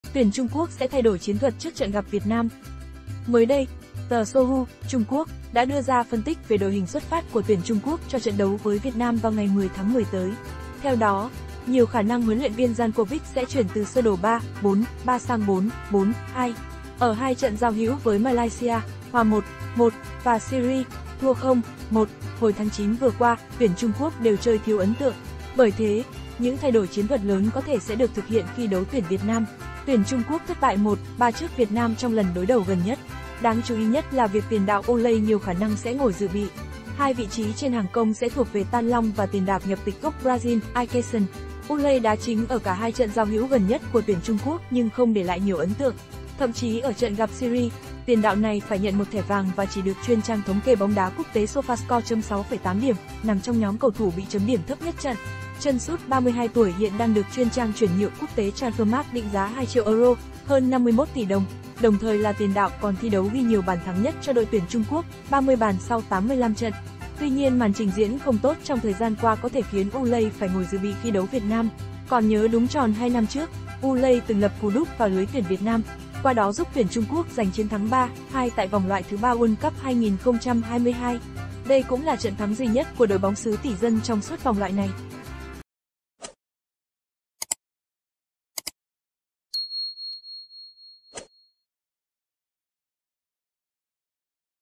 TUYỂN TRUNG QUỐC SẼ THAY ĐỔI CHIẾN THUẬT TRƯỚC TRẬN GẶP VIỆT NAM
Mới đây, tờ Sohu (Trung Quốc) đã đưa ra phân tích về đội hình xuất phát của tuyển Trung Quốc cho trận đấu với Việt Nam vào ngày 10/10 tới. Theo đó, nhiều khả năng  Huấn luyện viên Jankovic sẽ chuyển từ sơ đồ 3-4-3 sang 4-4-2. Ở 2 trận giao hữu với Malaysia (hòa 1-1) và Syria (thua 0-1) hồi tháng 9 vừa qua, tuyển Trung Quốc đều chơi thiếu ấn tượng. Bởi thế, những thay đổi chiến thuật lớn có thể sẽ được thực hiện khi đấu tuyển Việt Nam. Tuyển Trung Quốc thất bại 1-3 trước Việt Nam trong lần đối đầu gần nhất. Đáng chú ý nhất là việc tiền đạo Wu Lei nhiều khả năng sẽ ngồi dự bị. 2 vị trí trên hàng công sẽ thuộc về Tan Long và tiền đạp nhập tịch gốc Brazil, Ai Kesen. Wulei đá chính ở cả hai trận giao hữu gần nhất của tuyển Trung Quốc nhưng không để lại nhiều ấn tượng. Thậm chí ở trận gặp Syria, tiền đạo này phải nhận 1 thẻ vàng và chỉ được chuyên trang thống kê bóng đá quốc tế Sofascore chấm 6,8 điểm, nằm trong nhóm cầu thủ bị chấm điểm thấp nhất trận. Chân sút 32 tuổi hiện đang được chuyên trang chuyển nhượng quốc tế Transfermark định giá 2 triệu euro (hơn 51 tỷ đồng), đồng thời là tiền đạo còn thi đấu ghi nhiều bàn thắng nhất cho đội tuyển Trung Quốc (30 bàn sau 85 trận). Tuy nhiên màn trình diễn không tốt trong thời gian qua có thể khiến Wu Lei phải ngồi dự bị khi đấu Việt Nam. Còn nhớ đúng tròn 2 năm trước, Wu Lei từng lập cú đúp vào lưới tuyển Việt Nam, qua đó giúp tuyển Trung Quốc giành chiến thắng 3-2 tại vòng loại thứ 3 World Cup 2022. Đây cũng là trận thắng duy nhất của đội bóng xứ tỷ dân trong suốt vòng loại này.